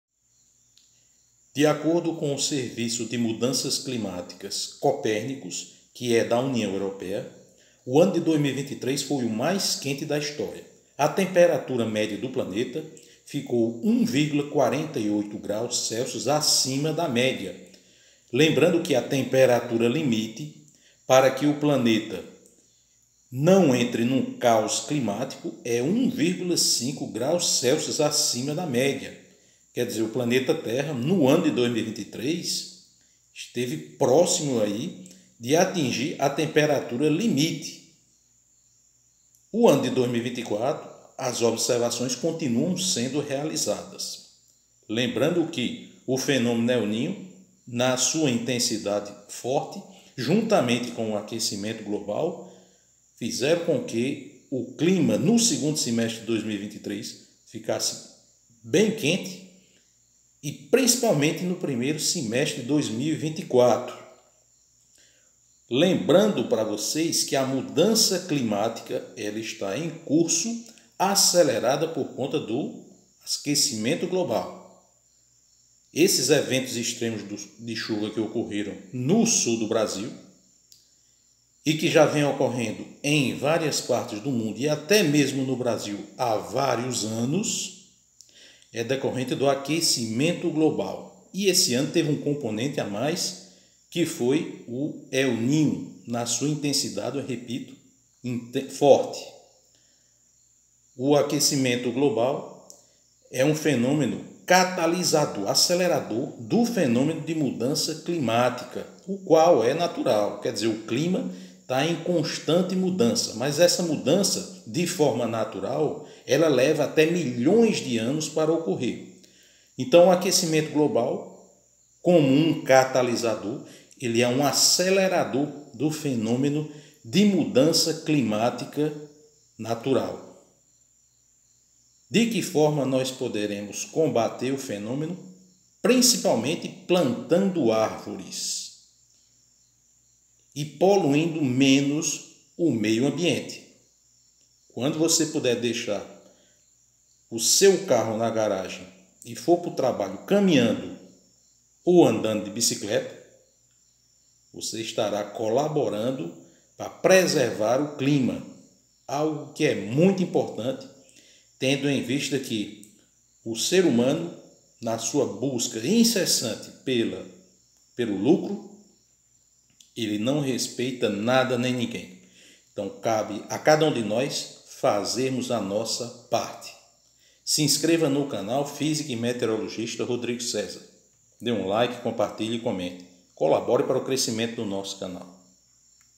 2023 foi o ano mais quente da História
De acordo com o Serviço de Mudanças Climáticas Copernicus da União Européia, o ano de 2023 foi o mais quente da História, aponta no vídeo o físico, meteorologista, mestre em Meteorologia e doutor em Física Rodrigo Cézar.

Em 2023 a temperatura média do planeta atingiu 1,48ºC acima da média, lembrando que a temperatura média limite para que o planeta não entre em caus climático é de 1,5ºC acima da média.

Dessa forma, o aquecimento global como catalisador do fenômeno de mudança climática natural é o grande vilão do calor em excesso e consequentemente, dos eventos extremos do clima que vêm sendo observados há vários anos no Brasil e em várias parte do mundo.

O fenômeno de mudança climática é natural, no entanto, o aquecimento global funciona como um catalisador (acelerador) do fenômeno de mudança climática natural, explica o estudioso.

Imagem de Tumisu por Pixabay